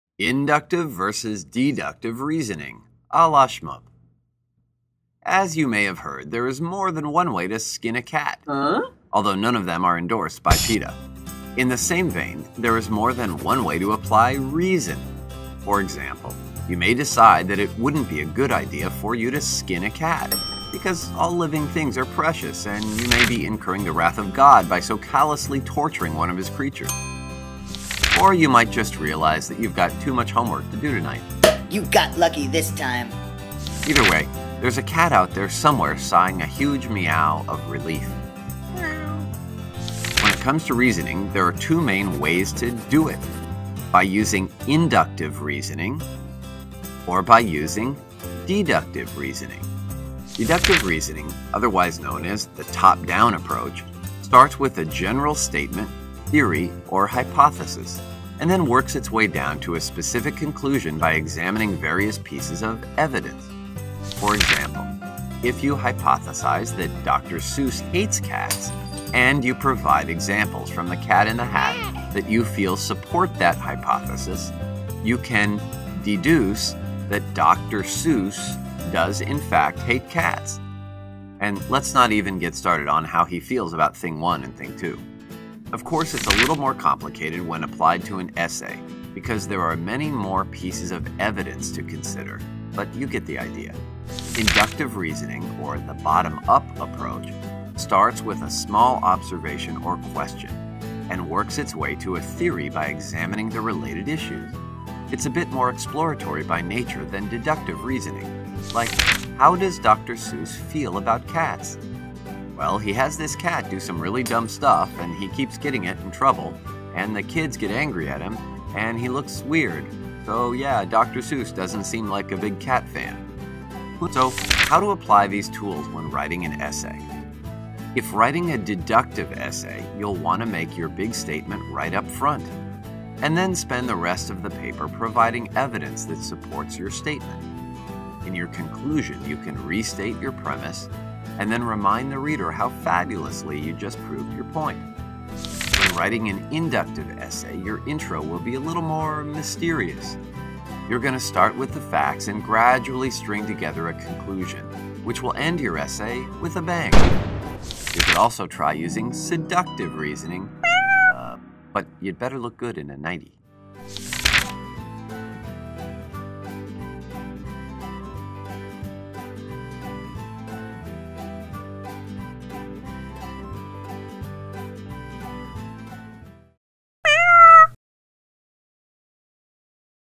Inductive VS Deductive Reasoning by Shmoop
There are two different ways to use reasoning: deductive and inductive. Deductive reasoning starts with a general theory, statement, or hypothesis and then works its way down to a conclusion based on evidence. Inductive reasoning starts with a small observation or question and works it's way to a theory by examining the related issues.